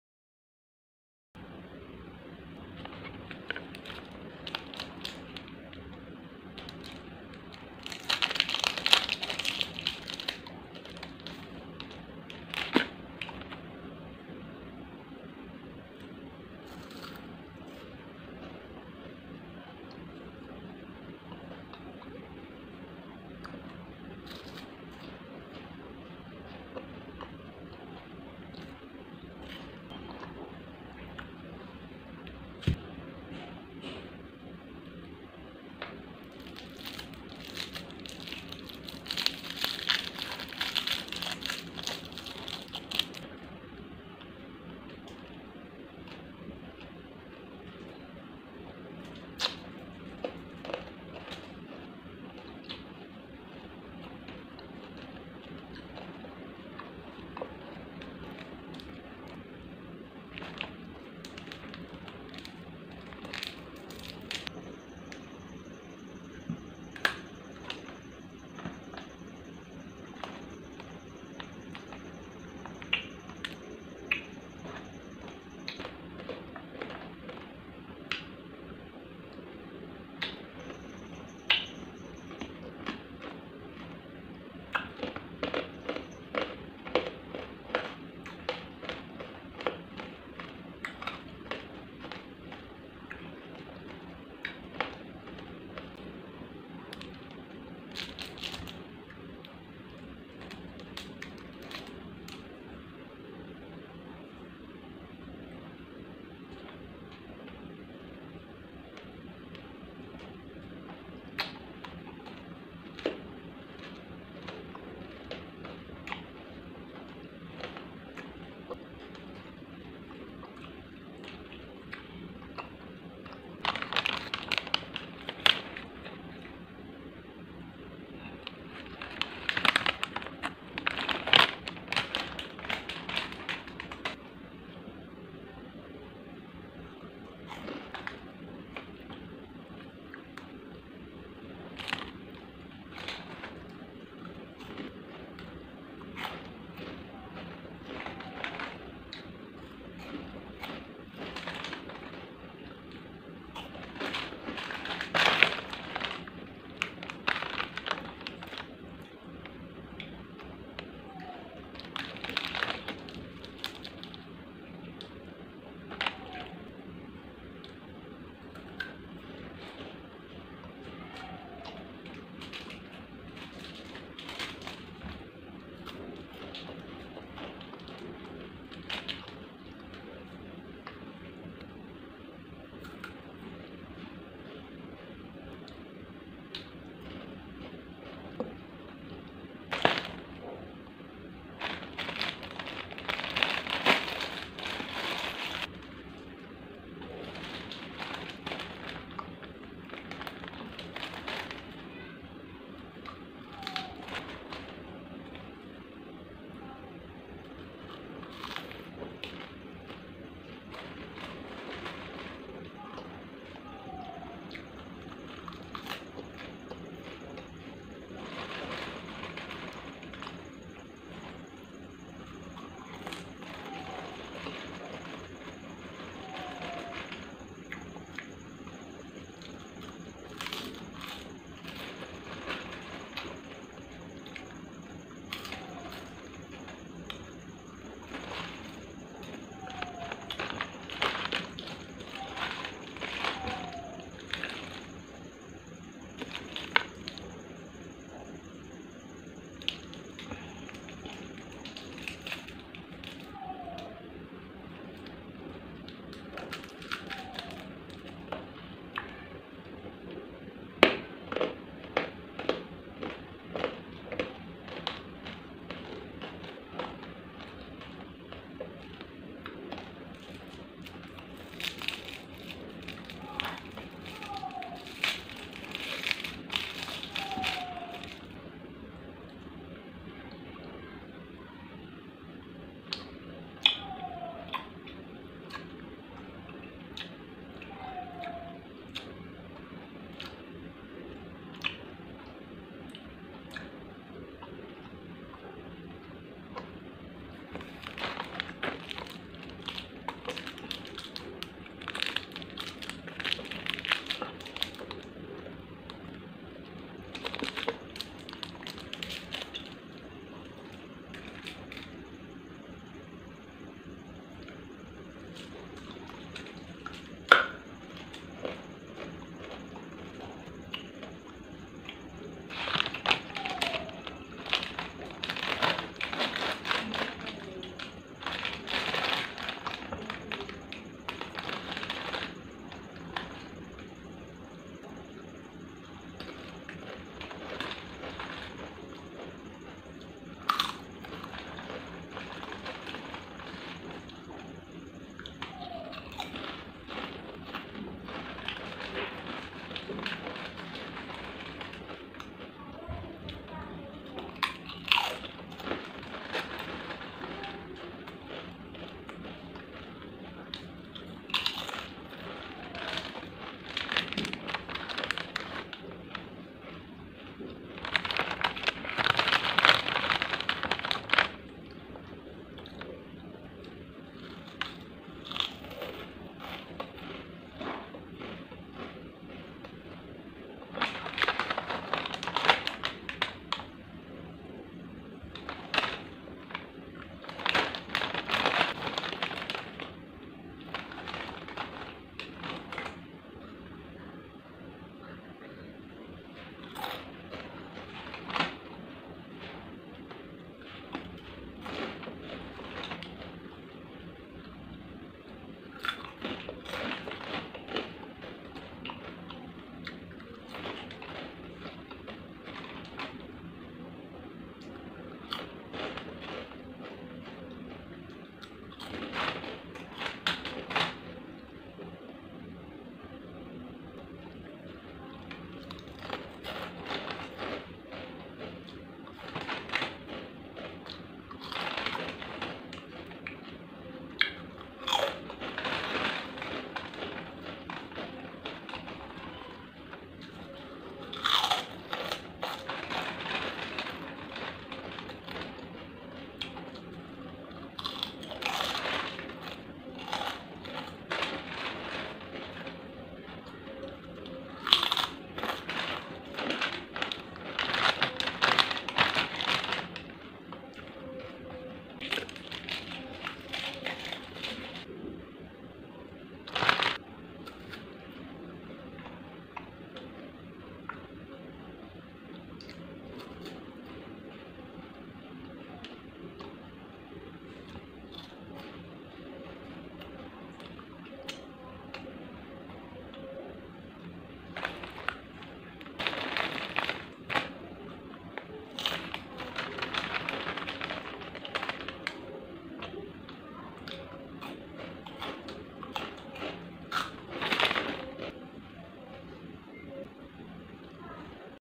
Asmr Eating Indian snack under Rs1to5 only 🙄 what You can eat in india within 5 rs. Vegan asmr;)
Today I am going to eat Indian snacks under Rs1to5 . What you can eat in india within 5 rs.
Very interesting video
Don't forget to check my other video on YouTube channel

💫My friend channel link